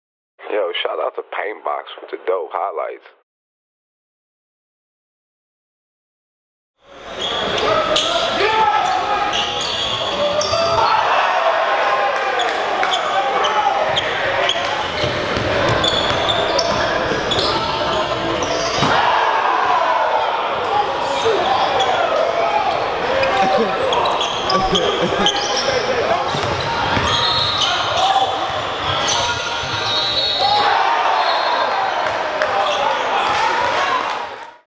Jayvon Graves has BOUNCE!!!
Jayvon Graves is SUPER ATHLETIC!!! Watch him throw down a couple dunks at the Brawl for the Ball tournament that was held in Grand Rapids, MI July 14th-17th 2016.